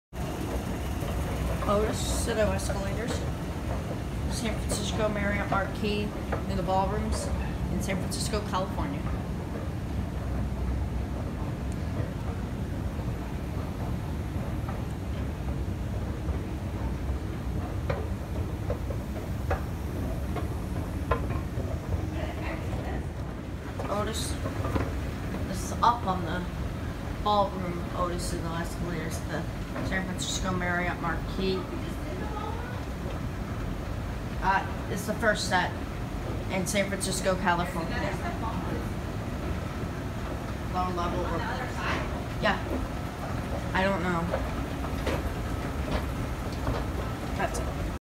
OTIS/SITO Escalators At San Fransisco Marriott Marquis San Fransisco CA
These connect the Lobby and the Upper Ballroom Level

Installed 1989